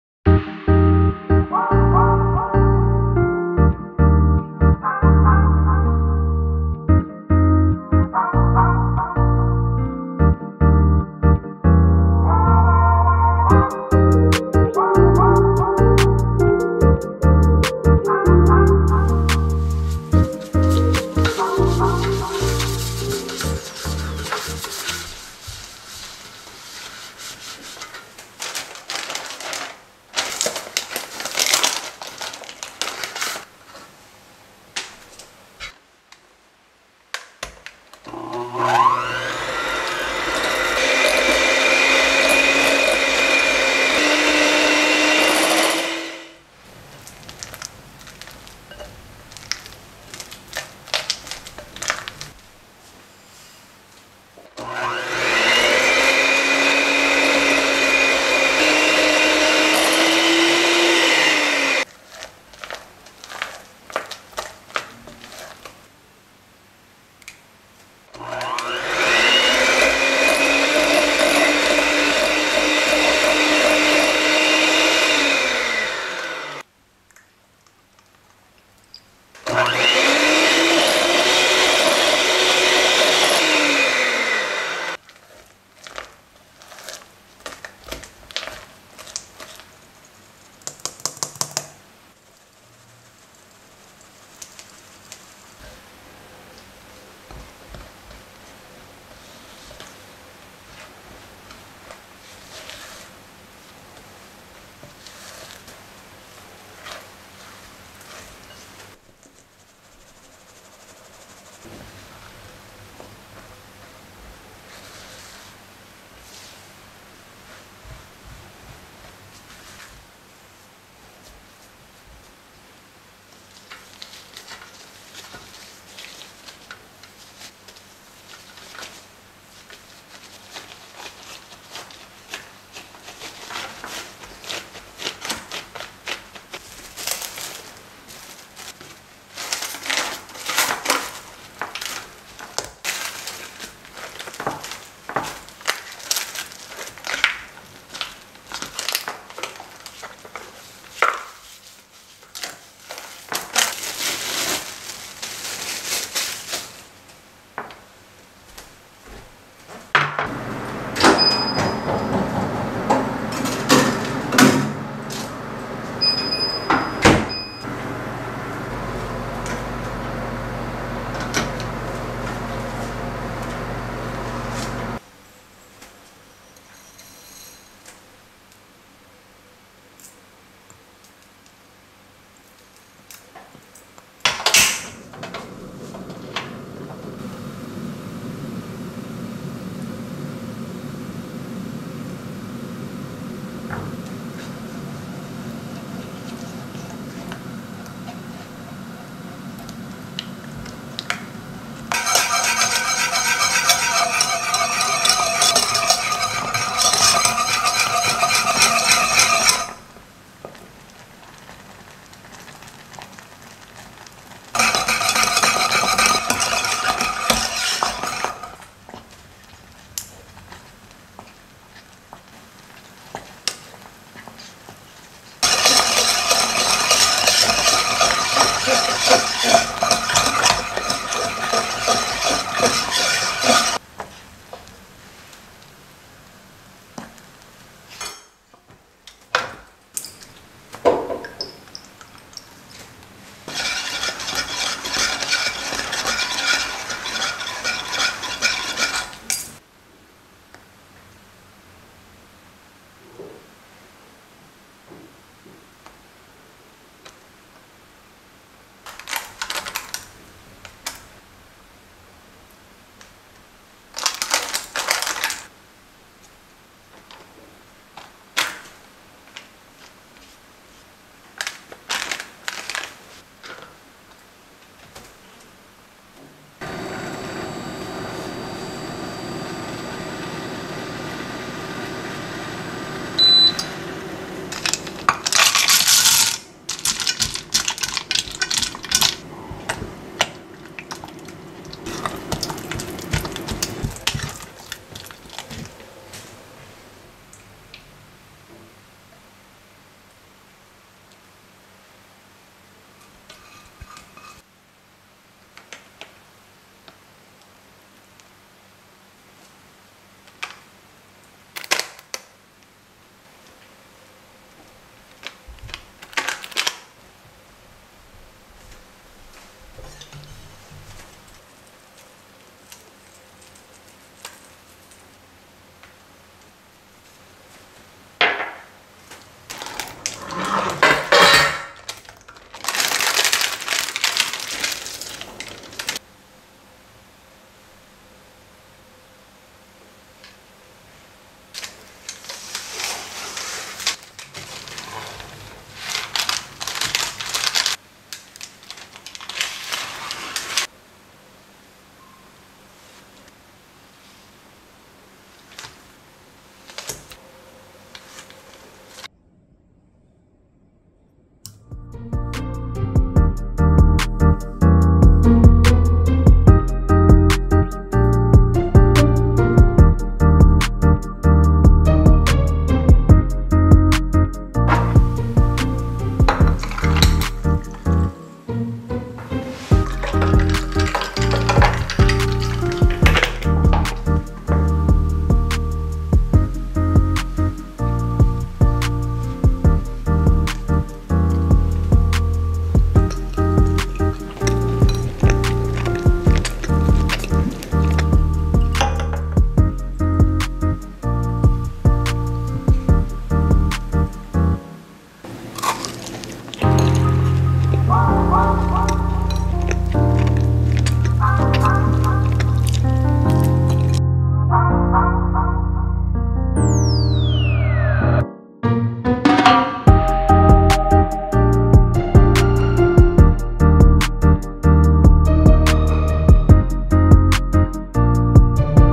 Salted Caramel Millionaires Shortbread | Caramel Squares | Snowuri Baking
This salted caramel shortbread is a buttery shortbread crust topped with chewy caramel and thick chocolate ganache. Additionally, sprinkling sea salt on top moderates the decadent sweetness of the gooey caramel and thick chocolate. If you take one bite, you will figure out where the name 'millionaires shortbread' comes from.